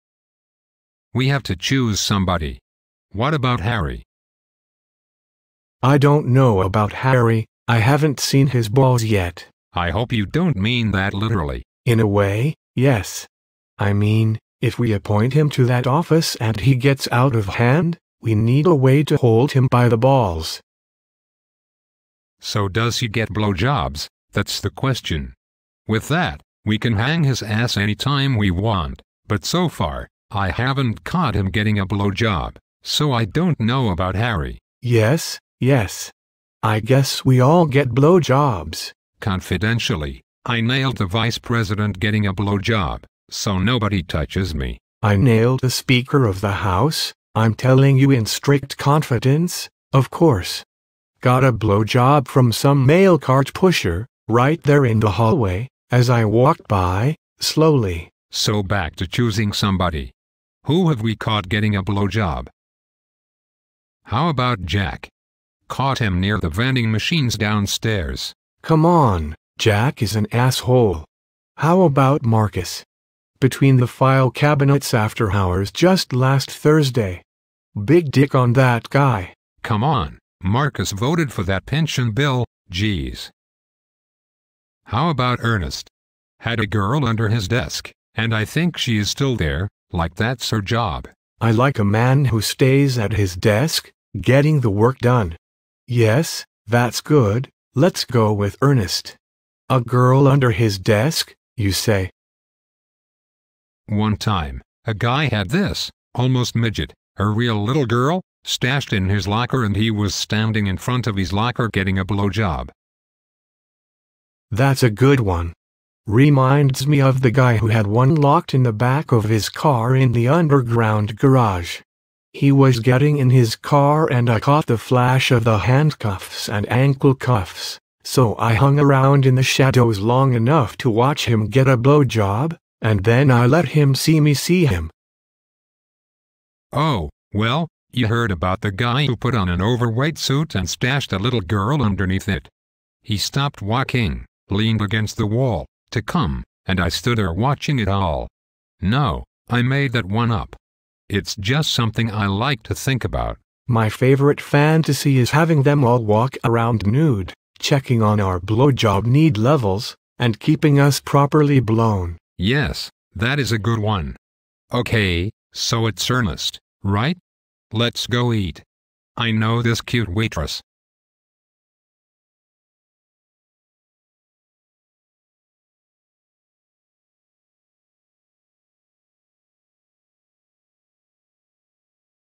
Sufi george listens in on a Senate sub-committee.
Fly on the wall Sufi George listens as senators talk more about blow jobs than about naming an appointee.